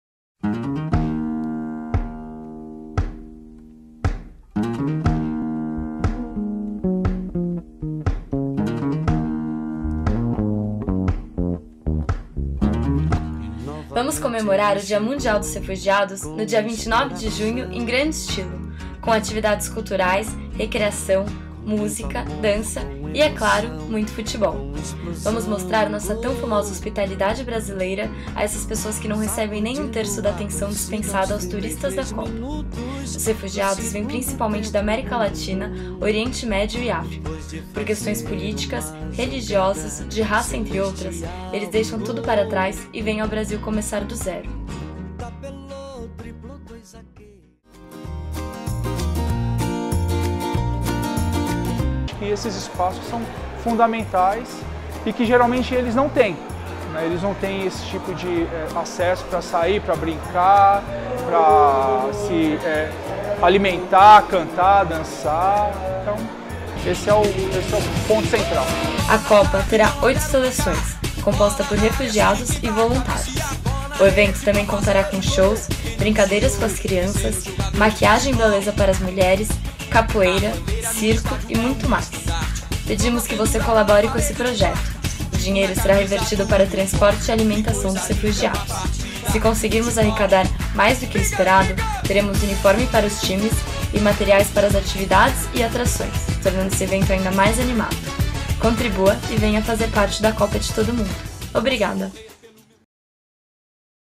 Copa dos refugiados 2014 2
A copa dos refugiados é um evento que surge com uma proposta de uma copa de todo mundo, feita por todos nós para celebrar o dia mundial do refugiado. Nos ajude a mostrar nossa famosa hospitalidade brasileira. Doe através do juntos e participe do evento!